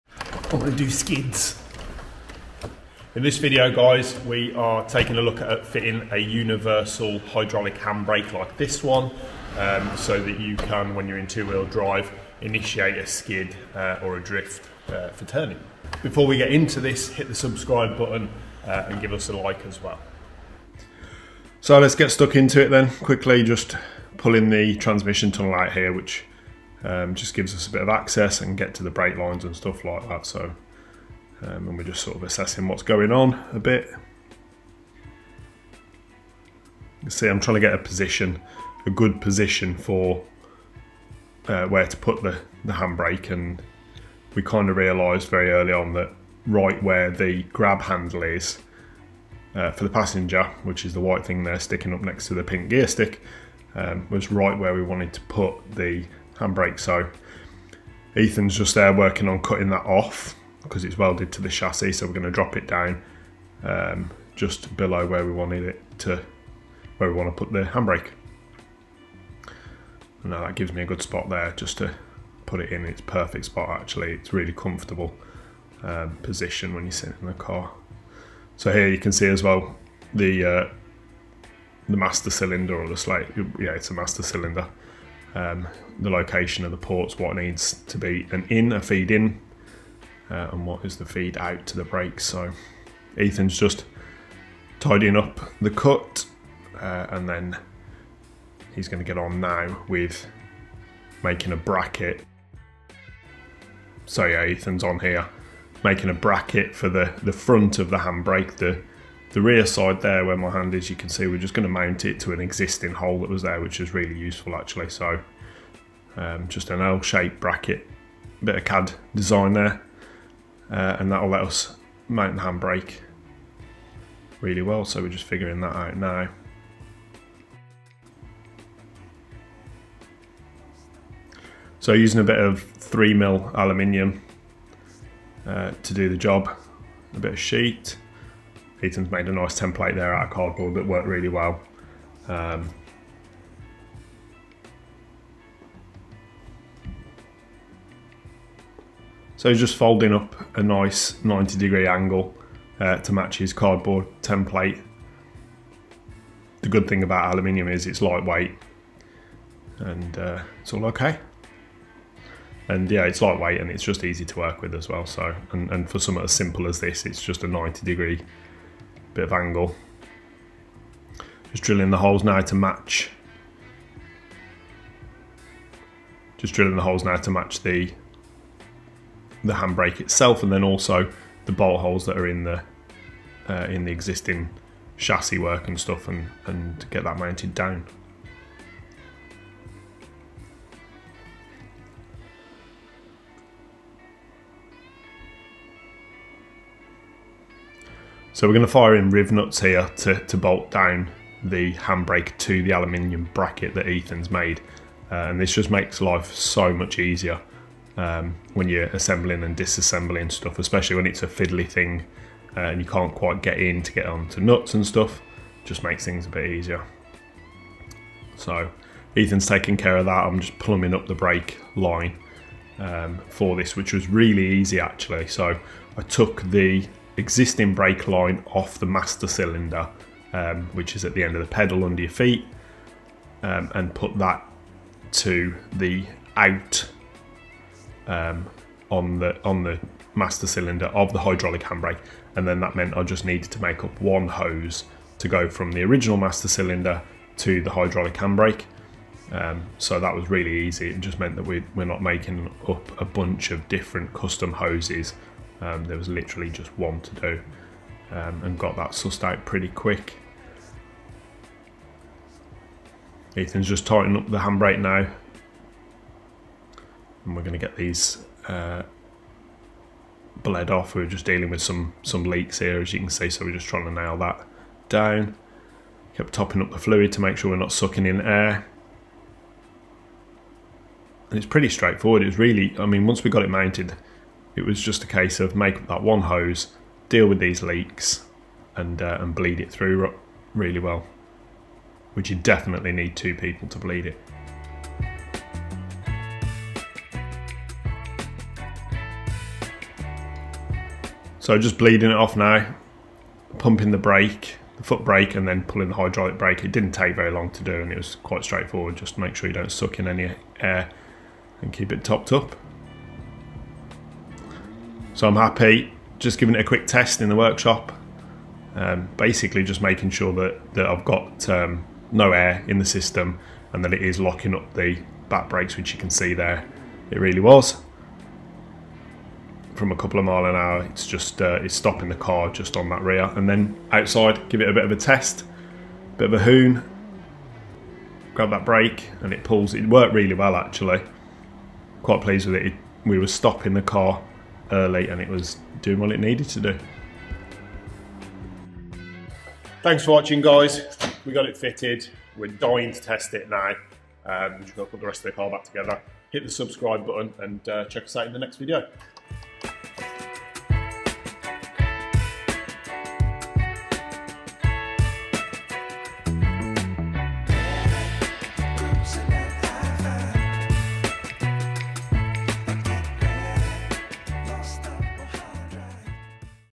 Fitting a Can Am Maverick X3 Hydraulic Drift Brake
Part of the 2022/2023 winter modifications on the Can Am Maverick X3 was a quick and easy installation of a Hydraulic Drift Brake!
Looking forward to skidding into the new race season 🚙💨🏁